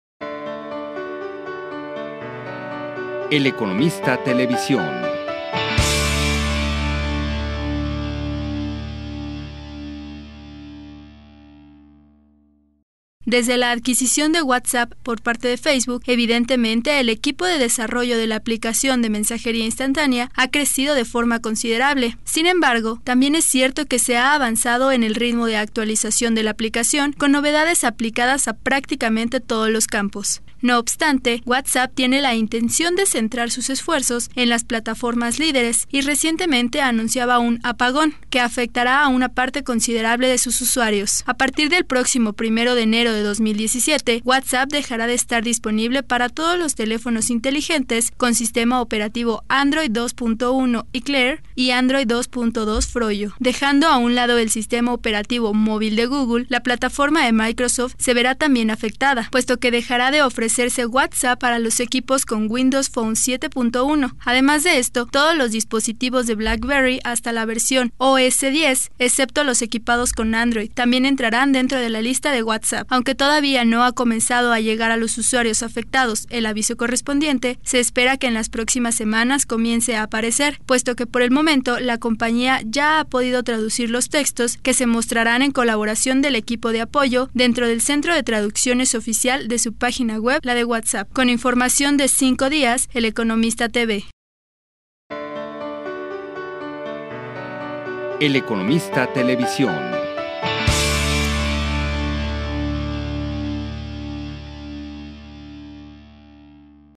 ¿A qué móviles afecta el apagón de WhatsApp?
Desde la adquisición de WhatsApp por parte de Facebook, evidentemente el equipo de desarrollo de la aplicación de mensajería instantánea ha crecido de forma considerable.